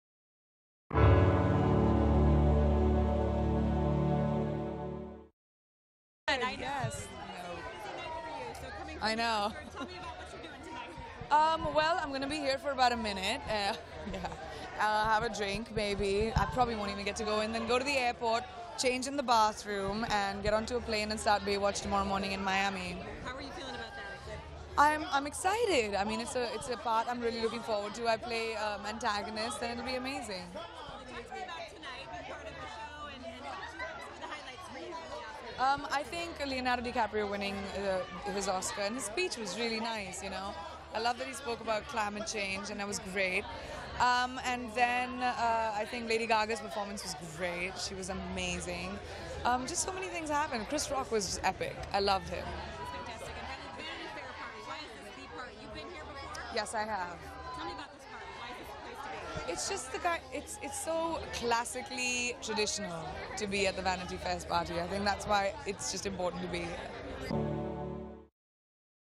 Priyanka Chopra Goes Sexy To Vanity Fair After Party
Priyanka Chopra looked incredibly sexy while dressed in while at the Vanity Fair after party which featured premiere guests once the @016 Oscars came to a close. She talks about what it means to attend the party as well as giving some info as to where she's heading next. It is also revealed that she will star in the new 'Baywatch'.